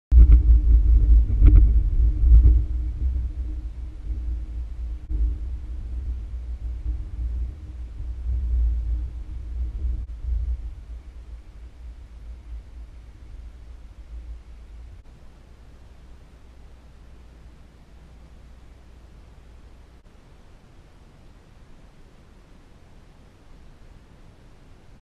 LIGHTNING STORM at night
Lightning storm that went by here in Tennessee. Pure amazingness. There was about one strike every five to thirty seconds. Recorded with GoPro Hero Plus.